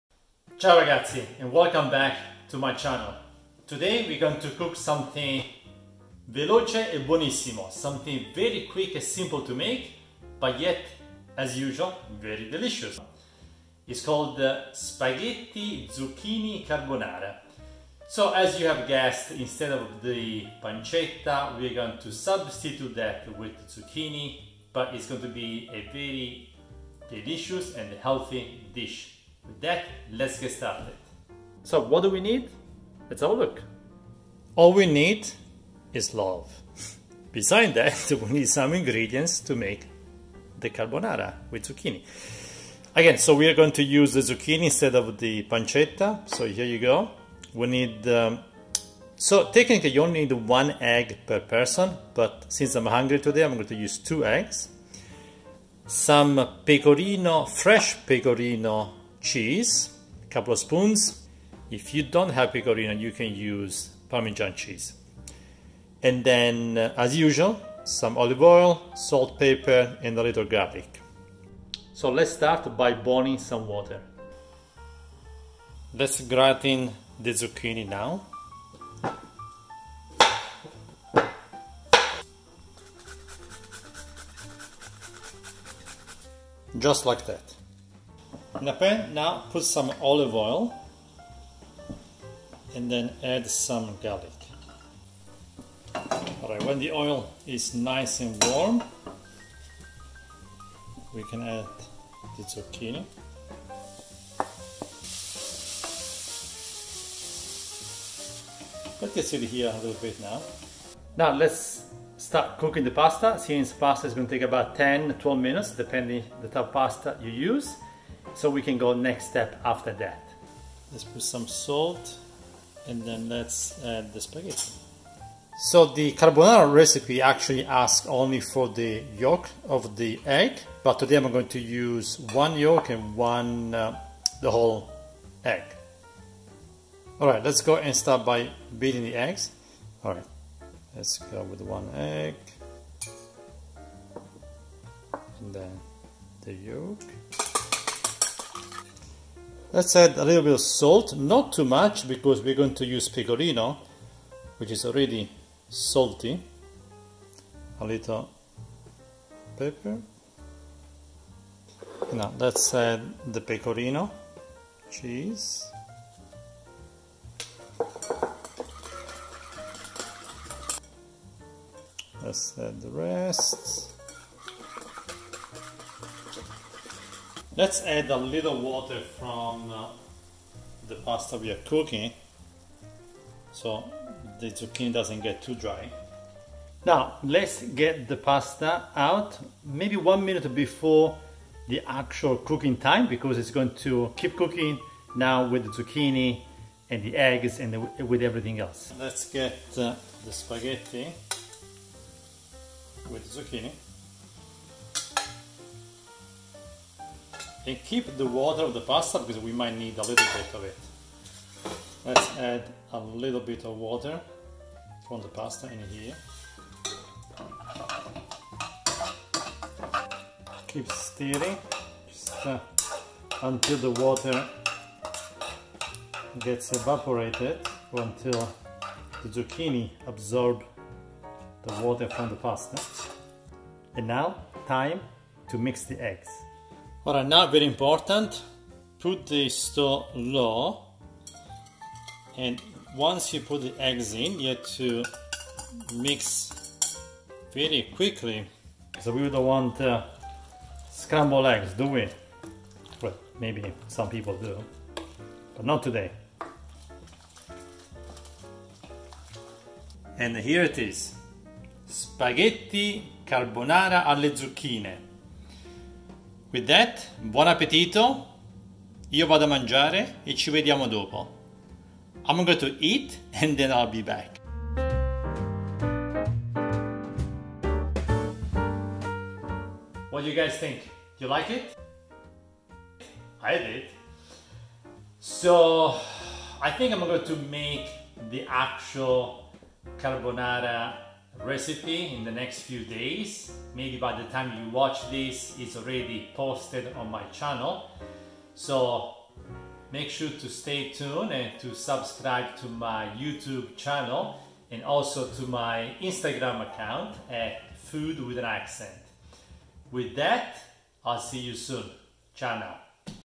Amazing Vegetarian CARBONARA Recipe - Only 4 Main Ingredients for Dinner
Only 4 Main Ingredients for an Amazing Vegetarian Carbonara Recipe - This amazing vegetarian spaghetti carbonara is only with zucchini, eggs, and cheese, a very healthy and delicious pasta dish for  lunch or dinner.

With the latest wave of veganism and vegetarianism in Italy, so many classic and traditional recipes have been revised and varied. So, for example, on the basis of the classic Roman Carbonara, this dish was born, in which the pork cheek "guanciale" was replaced by zucchini. Perfect summer cooking, light, delicate and appetizing.

Ingredients for 2 servings:
• 2 Zucchini
• 3 Eggs (2 yolks and 1 whole)
• 200g (0.4 oz) Spaghetti
• 2 tbsp Pecorino Cheese

• Olive Oil, Salt, Pepper, and Garlic

Instruction:
1. Boil some water in a skillet and grate the zucchini
2. Put some olive oil in a pan and fry the garlic in it for 20 – 30 seconds. Don't make it brown.
3. Now add zucchini to the pan and let it cook on low flame.
4. Put some salt in the boiling water and add spaghetti to it.
5. Beat 2 yolks and 1 egg white in a small bowl and add a little bit of salt, pepper, and Pecorino Cheese in it. Mix them well.
6. Take a cooking spoon of water from the pasta and add it to the zucchini. It will not let the zucchini too dry.
7. Now Mix the spaghetti with the zucchini and add a little bit of water. Keep stirring.
8. Lower the heat and mix the egg. Keep mixing the eggs, and don't let them overcook here.
9. Your Vegetarian Spaghetti Carbonara With Zucchini is ready. Serve hot!

Useful Tips:
• Add less salt because the Pecorino Cheese is already salty.
• Cook the pasta 1 to 2 minute less than the cooking time because you will cook it with zucchini as well.
• Sprinkle with Pecorino cheese, a drop of oil, and a sprig of parsley if desired.
• The egg cream should turn into an enveloping creamy mass but not an omelet.